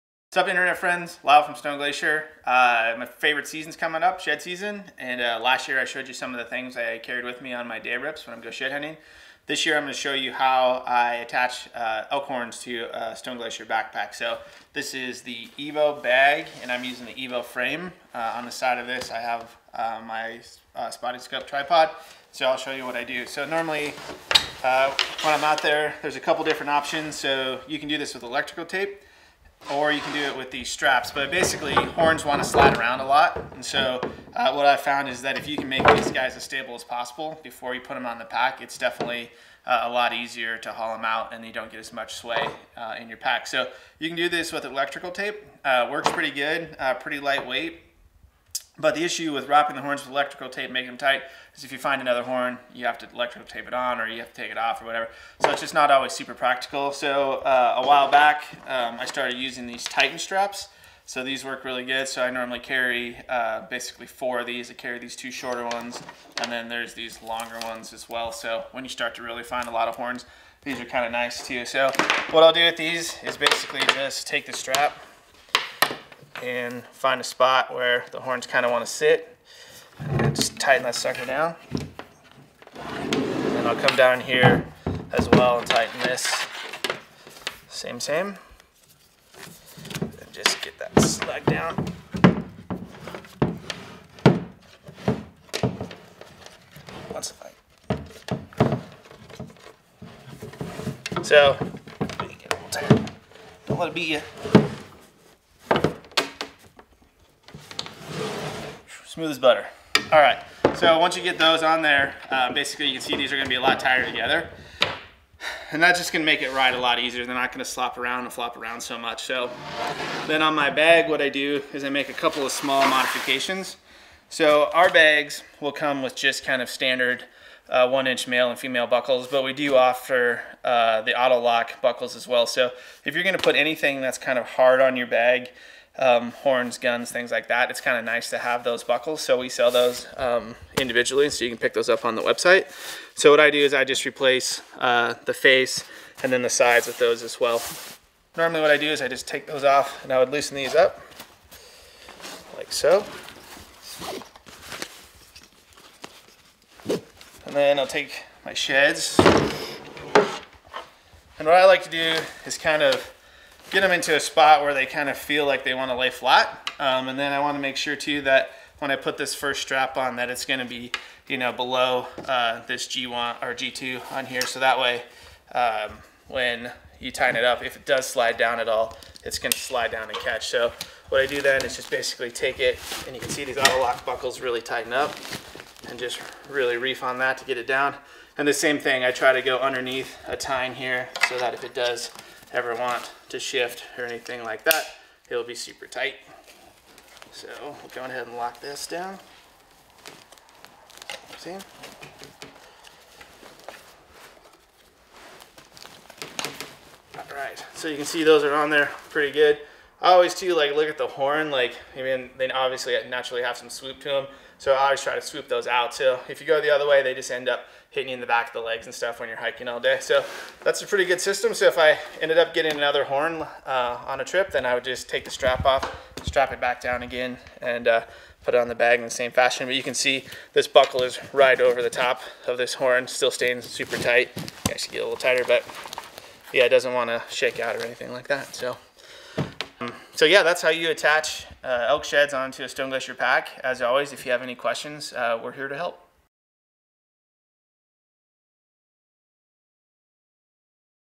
Elk Shed Attachment
Stone Glacier Marketing Manager, Lyle Hebel (@lylehebel) talks us through his method of attaching elk antler to his SG pack.  Learn more about our pack systems here

Video By Sam Averett (@samaverett)